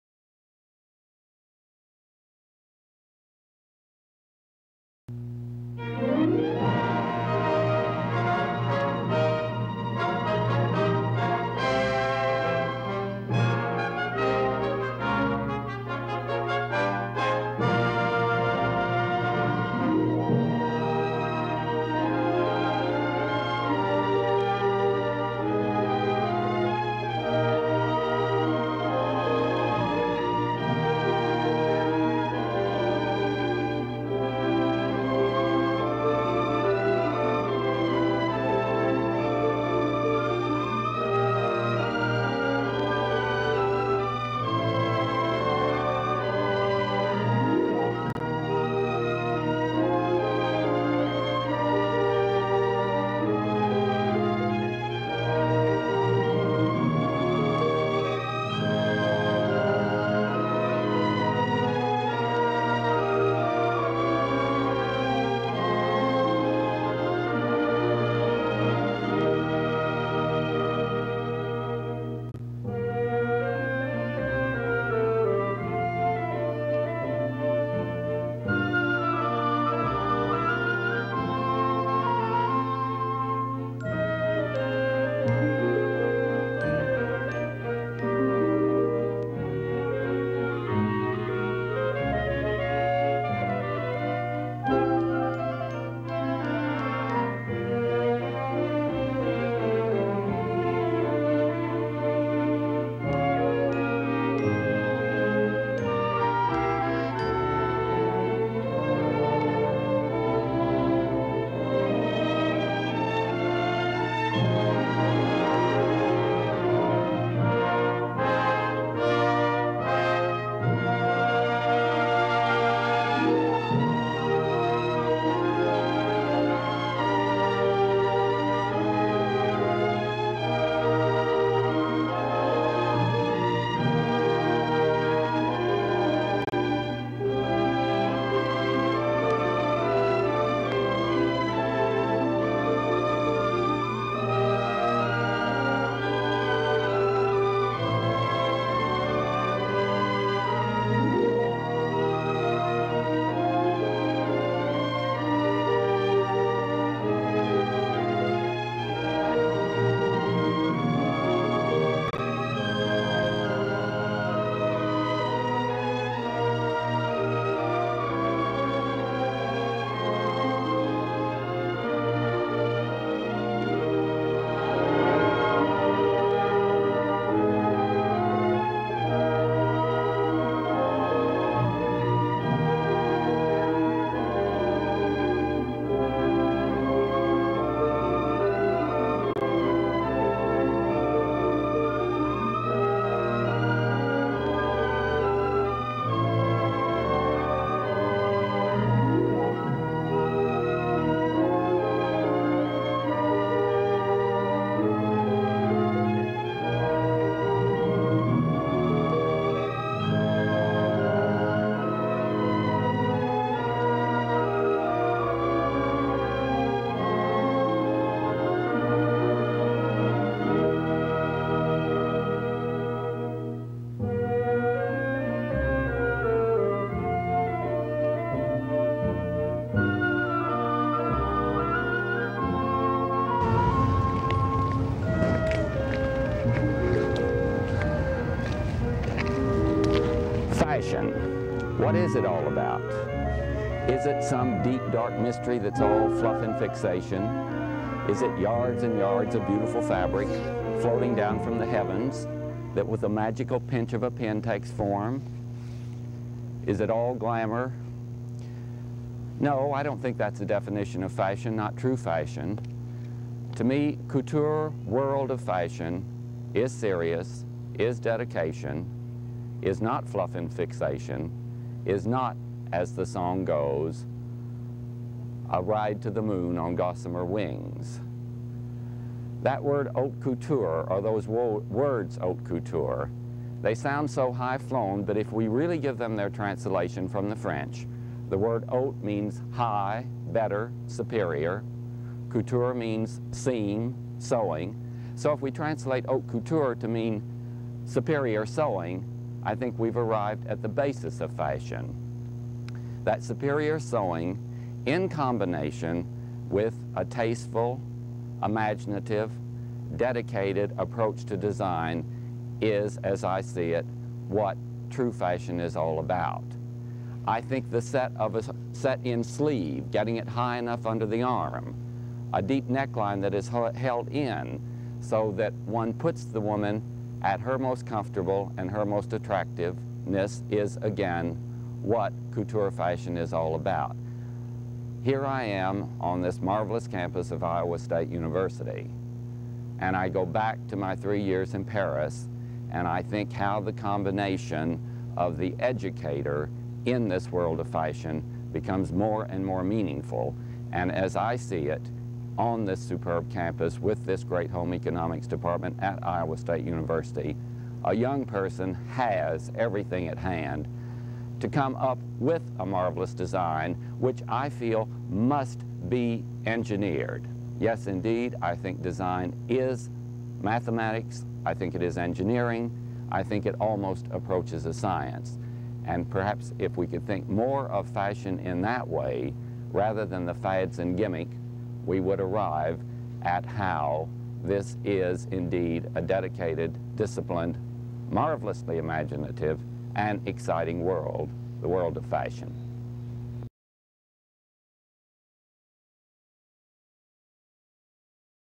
Couture Close-Ups with Charles Kleibacker, 1978
Produced by the Iowa State University Extension Service. New York fashion designer Charles Kleibacker visits Iowa State University to conduct a seminar on textiles and clothing. A number of dresses are modeled and Kleibacker provides his definition of fashion.